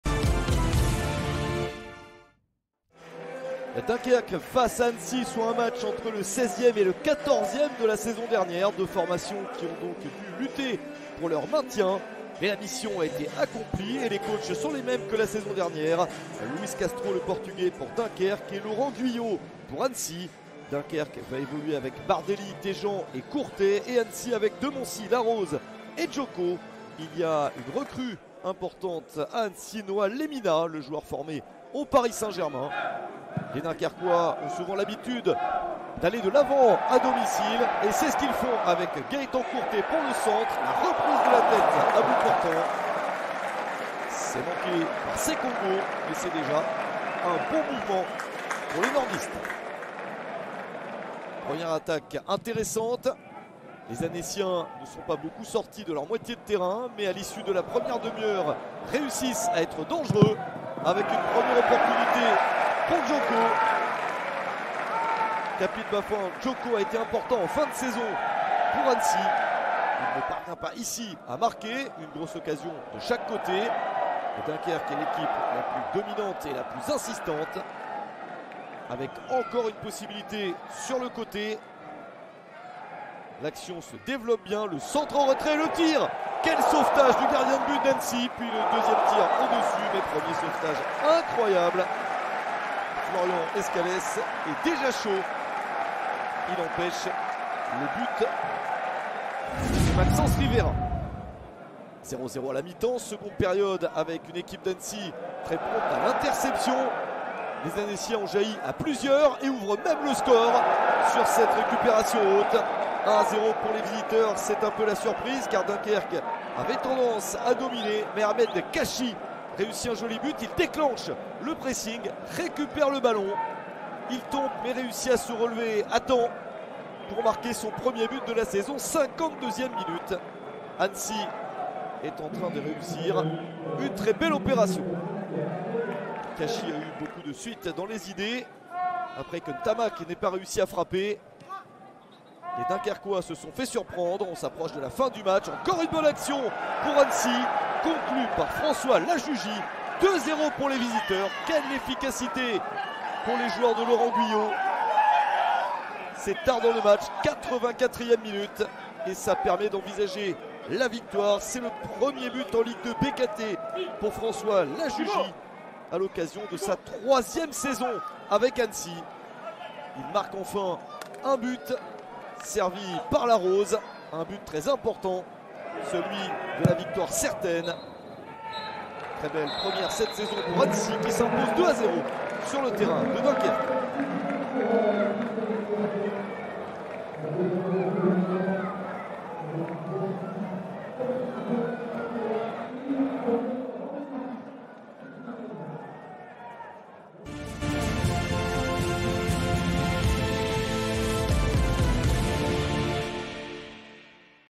USL DUNKERQUE - FC ANNECY (0-2) - 1ère journée - Ligue 2 BKT 2024-2025
Revivez les meilleurs moments de USL DUNKERQUE - FC ANNECY (0-2) en vidéo.

Ligue 2 BKT - Saison 2024/2025 - 1ète journée - Vendredi 16 Août 2024